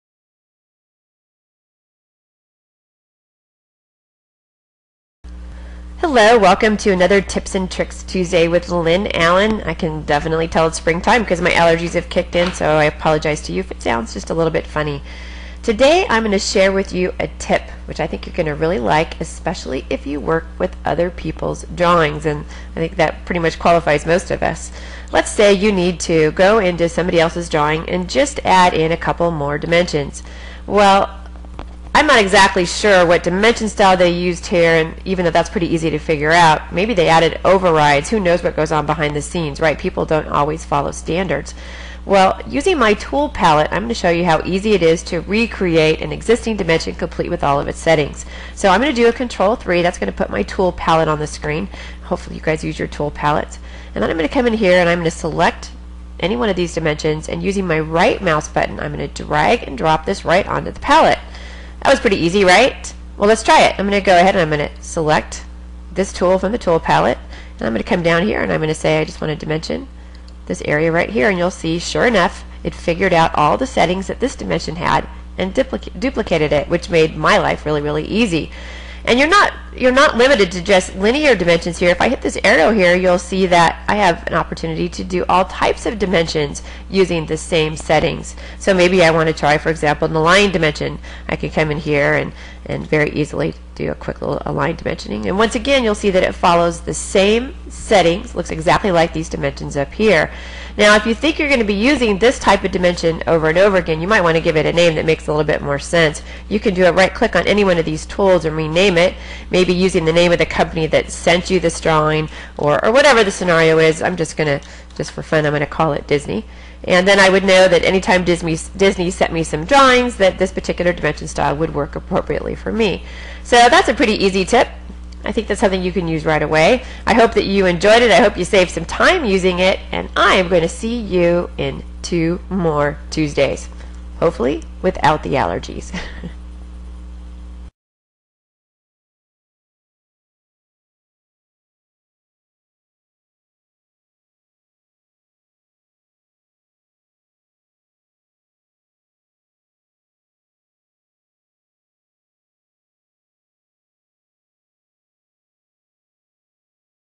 AutoCAD Tip -- Add Dimensions Shortcut (Lynn Allen/Cadalyst Magazine)
Adding dimensions to someone else's AutoCAD drawings can be frustrating because you aren't aware of all their settings!  This speedy trick makes it easy for you to quickly add dimensions that match perfectly. (AutoCAD 2002 and later) For more CAD tips,